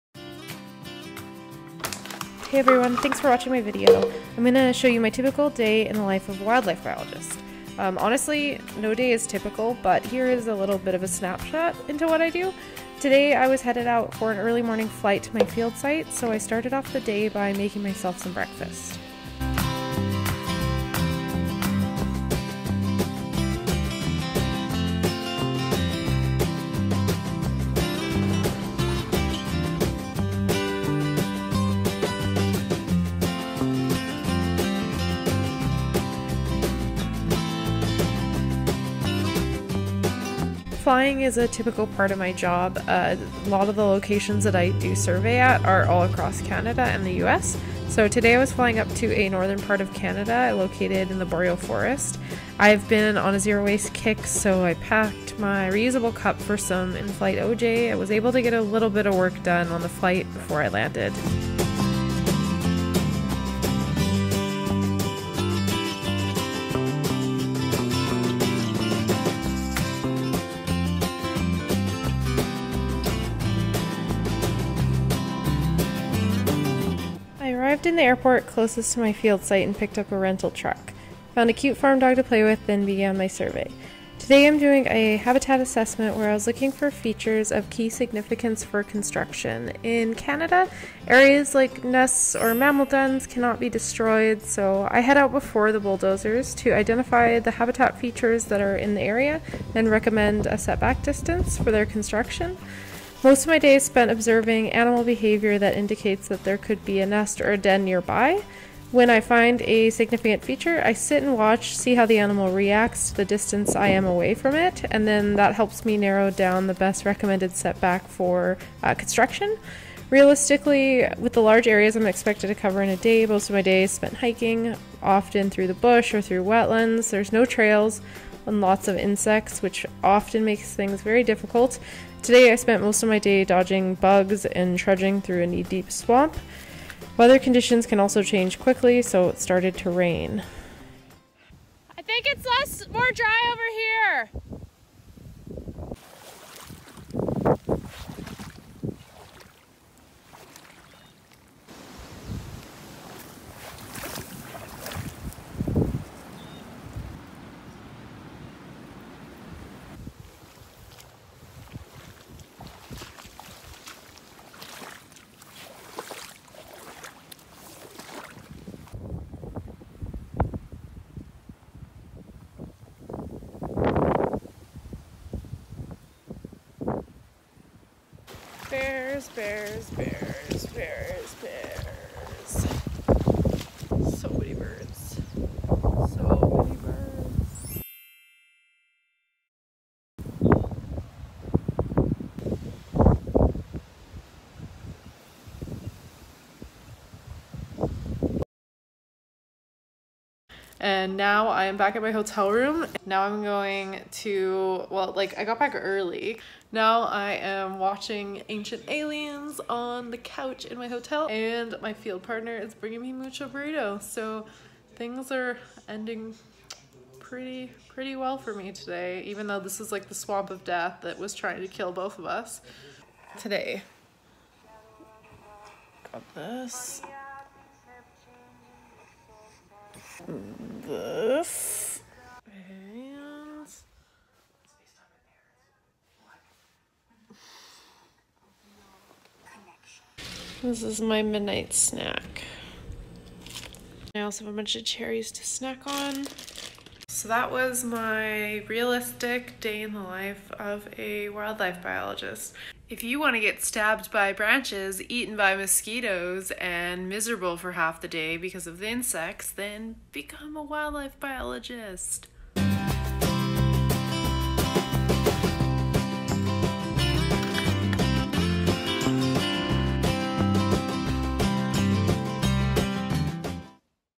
(HONEST) Day in the life of a Wildlife Biologist
Take a peek into my life as a wildlife biologist. I work for a consulting company doing wildlife habitat assessments, Species at Risk surveys, and assessing the impact of new developments on wild animals.